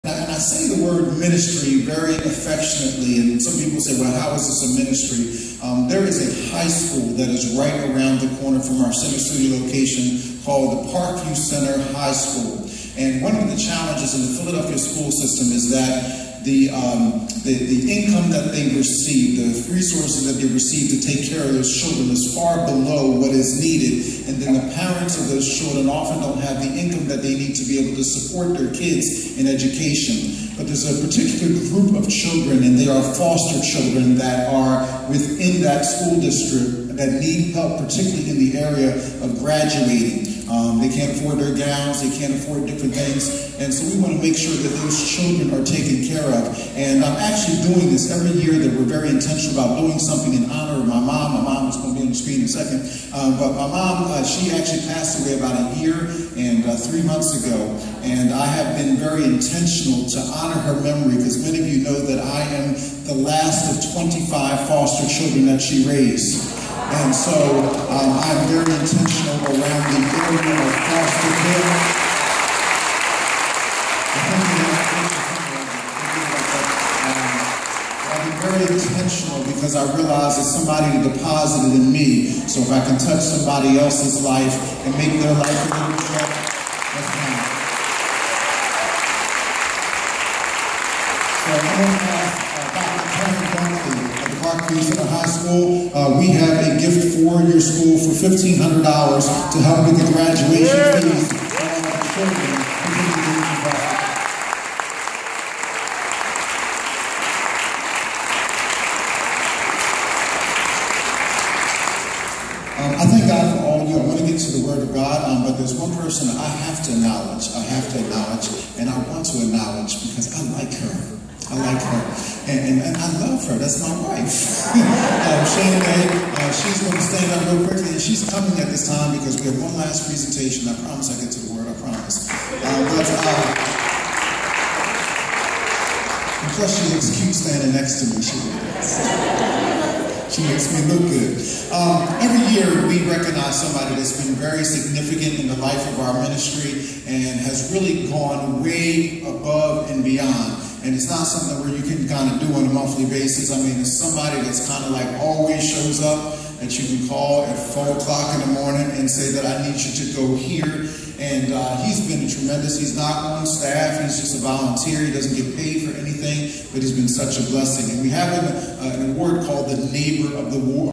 Chosen 300 helps foster children
Chosen 300 helps foster children at Parkview center high in honor of Brians foster mother.